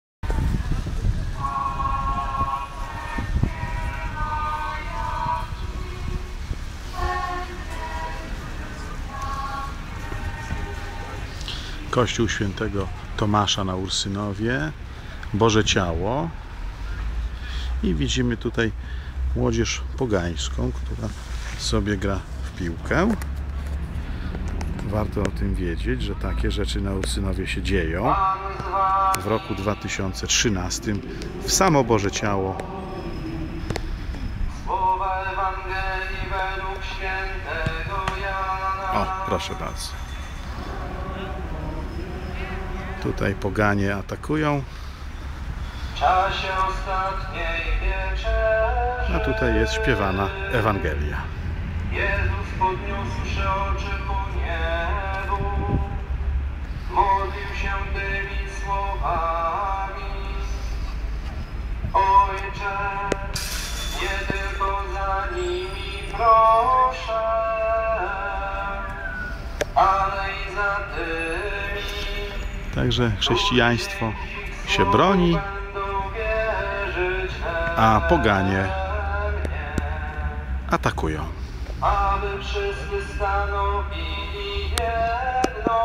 ATAK POGANSTWA PODCZAS PROCESJI BOZEGO CIALA NA URSYNOWIE
Film z 30-go maja 2013, około godziny 14:00. Scena chwytająca za gardło i dająca do myślenia. Podczas procesji Bożego Ciała, gdy Ksiądz Tomasz śpiewa Ewangelię za płotem u zbiegu ulic Gandhi i Dereniowej, nieopodal kościoła jacyś młodzieńcy grają w najlepsze w piłkę nożną. Szczyt chamstwa, brak szacunku dla kultury narodowej, złośliwa prowokacja, czy bezmyślność? Tyle się mówi o pogaństwie zalewającym Europę i proszę - prawdę się mówi!
Mi się najbardziej wydaje przykry fakt, że ludzie utracili wyczucie - jakie zachowanie jest pogańskie, a jakie nasze, chrześcijańskie. Bo granie w piłkę nożną gdy obok przechodzi procesja Bożego Ciała jest ewidentnie aktem postawy pogańskiej. Akt takiej postawy wydarzony na Ursynowie, w dzielnicy Warszawy która jest w bardzo dużym odsetku zamieszała przez dzieci i wnuki ubeków i esbeków oraz masonów warszawskich - ma dodatkowy smaczek sensacyjny. Zauważyłem to idąc w procesji, to było olsnienie - i dlatego spontanicznie wyciągnąłem kamerę, sfilmowałem 86-sekundowy film, umieściłem w internecie i... gmach Wielkiej Ubeckiej, Esbeckiej i Masońskiej Warszawy doznał kolejnej rysy. Dlatego ONI się tak pieklą - tam na FF-rondzie i na wielu forach internetowych. ONI czują, że ten ICHNI gmach się musi kiedyś zawalić, bo Bóg tak chce. Zaczęło się od Krzyża przed pałacem namiestnikowskim wzniesionym przez masonów, a skończy na statule Maryi na czubku Pałacu Kultury i Nauki wzniesionym przez Stalina.
+ Amen!